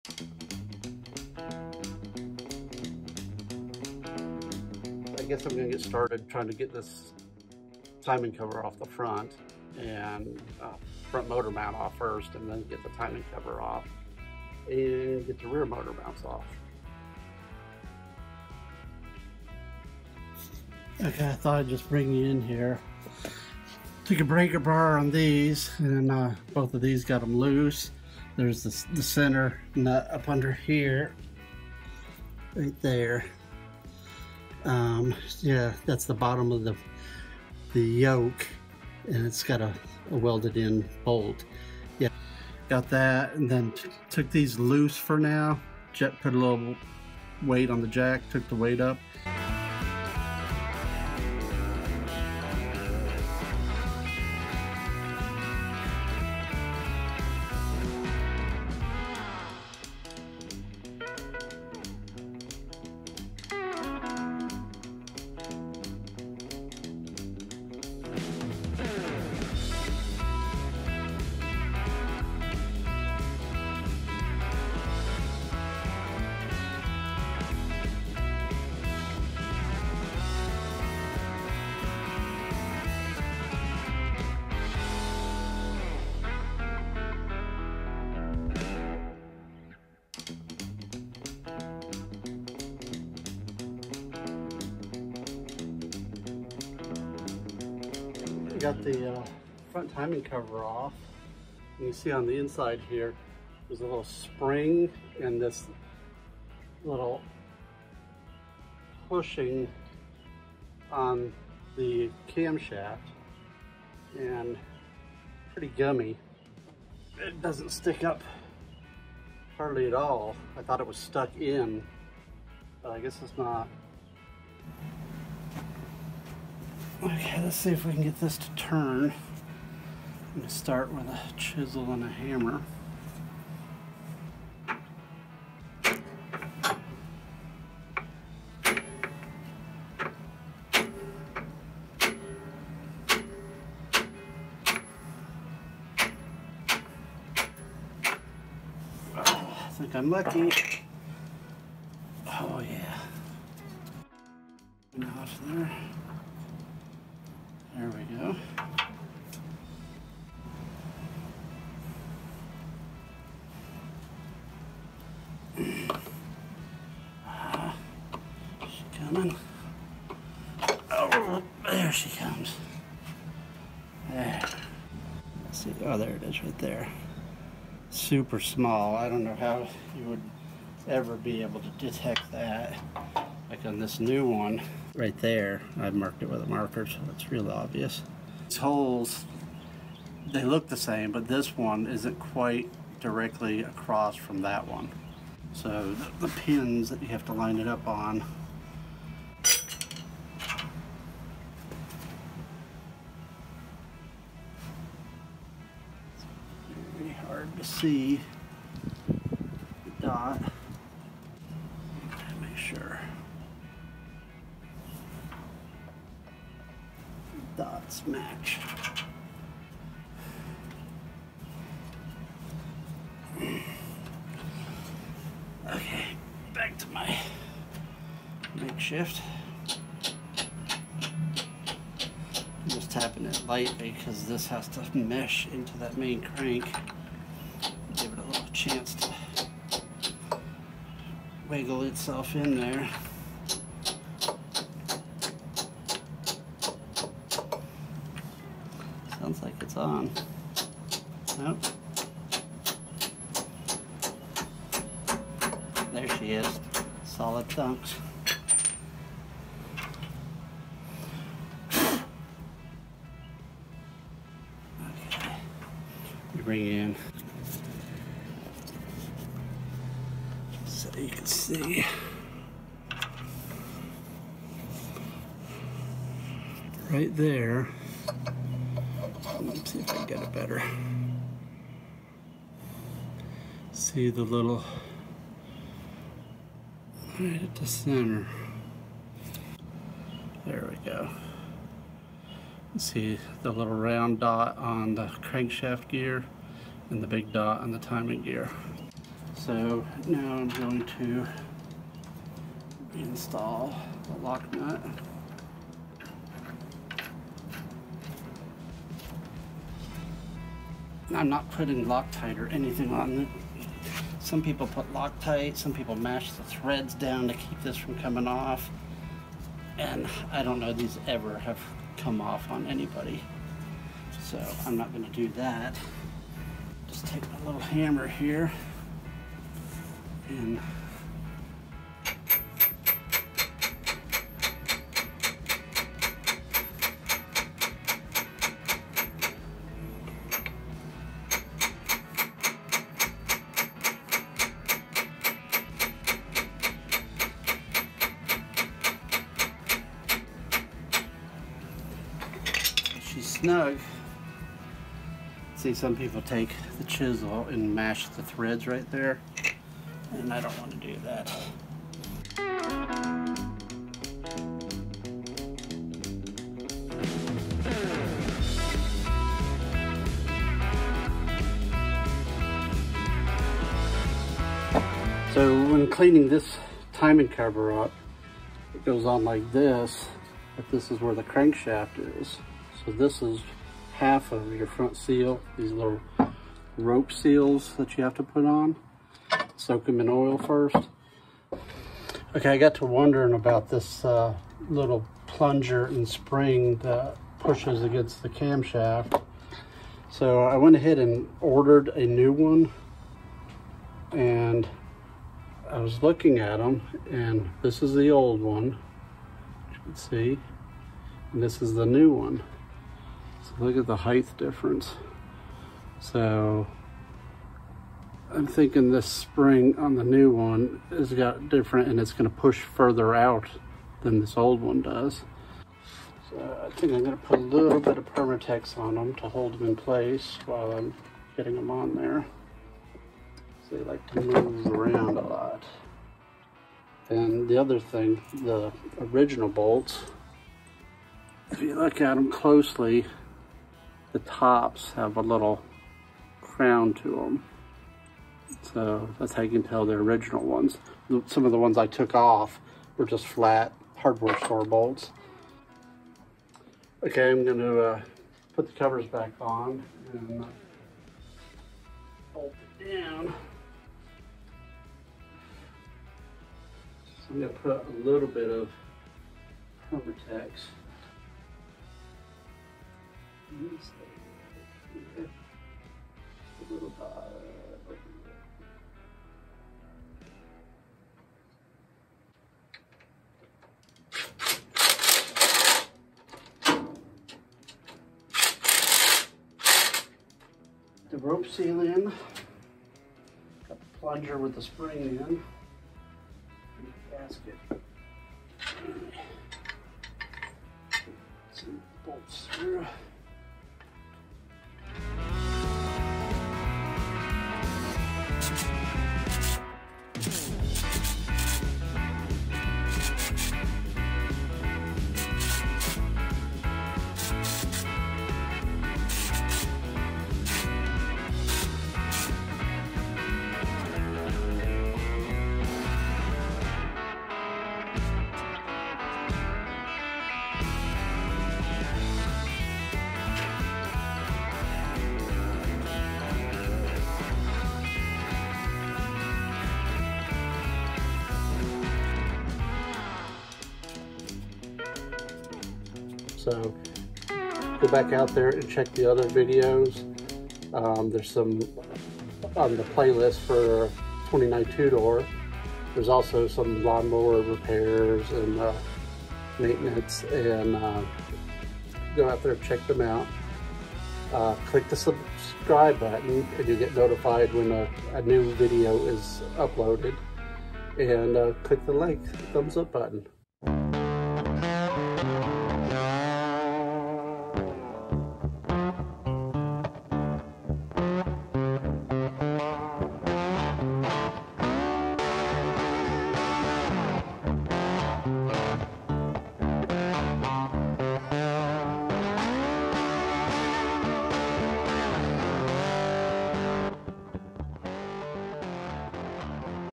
Ford Model A How-to: Replace Timing Gear on Model A fordmodela
How to replace the timing gear on a Ford Model A engine.

There are timing gears made of various types of materials, ie pressed fiber (original), aluminum, brass, etc. I decided to use the original pressed fiber.  Nevertheless it’s the same steps, which ever one you choose.

Note: I already had the radiator off, so this is different than a vehicle with the radiator on. This made it much easier to film/video.  The gear came off relatively easily. You might have to use a puller to remove the gear, and may have to remove your radiator to have better access.

The Model A suppliers sell a special tool to enable you to remove the camshaft timing gear retaining nut using a 1/2” ratchet or breaker bar. Mine came loose rather easily without damaging the camshaft nut.
If leaving the radiator on or if the nut is “stubborn”, look into the special tool. If it still will not come off, carefully apply heat to “break it loose”…. Ie. rusted in place, or if someone used Loctite on it.

I also needed to replace the engine motor mounts (front and rear), so I did that at the same time. That isn’t necessary, but should be looked into and if needed is a good idea to replace them while there.

so check them out..

Ted

NOTE:  I add CC (Closed Captioning). It takes a lot of time editing.. so if you have trouble hearing what is being said, turn on the CC on the YouTube app.